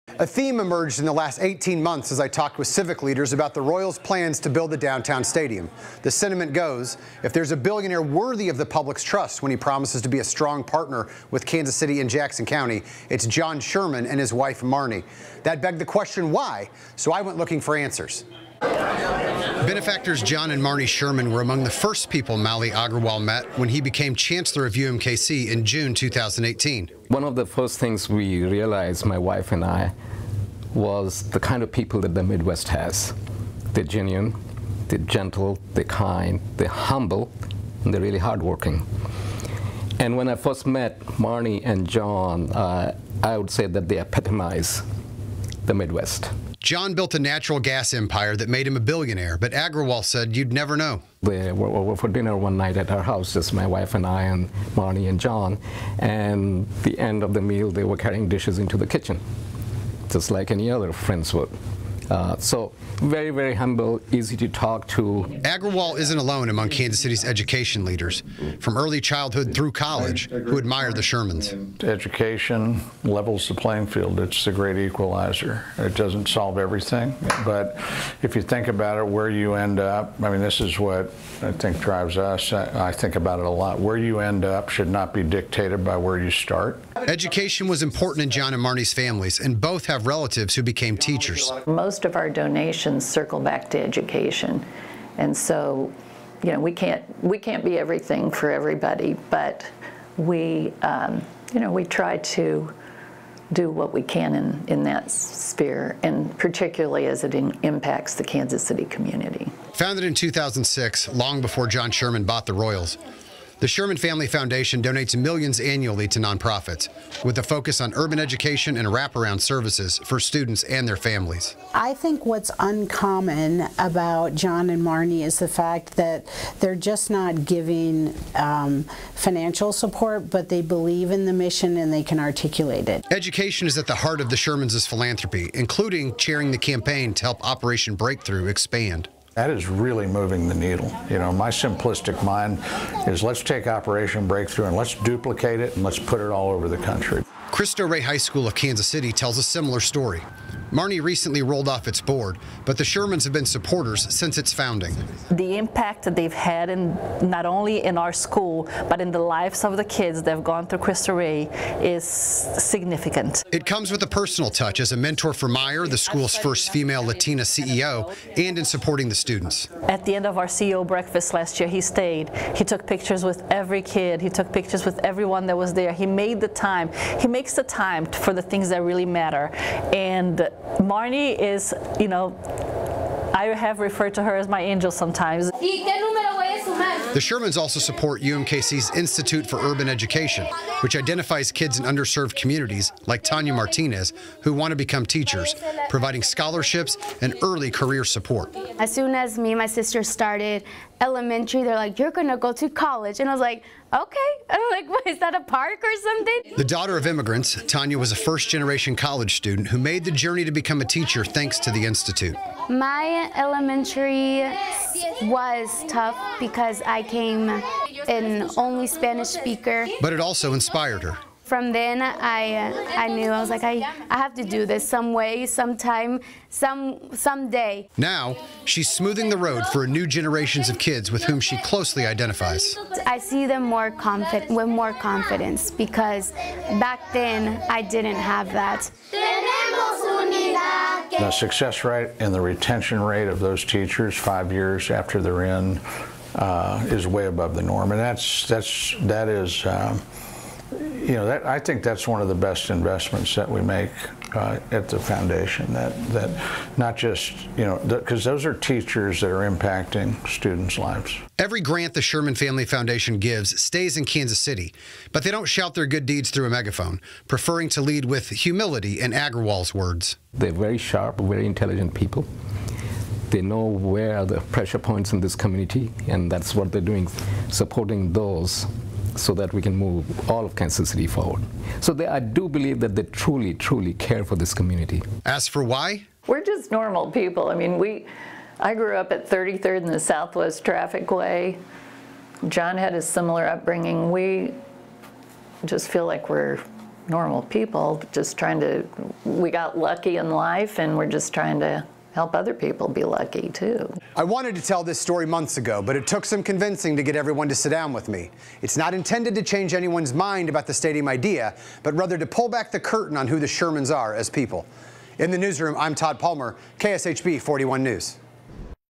'Billionaire you can trust': John and Marny Sherman build legacy through education, philanthropy
Kansas City Royals Chairman and CEO John Sherman and his wife, Marny, have quietly built a reputation of generosity and civic-mindedness, especially in supporting urban education efforts.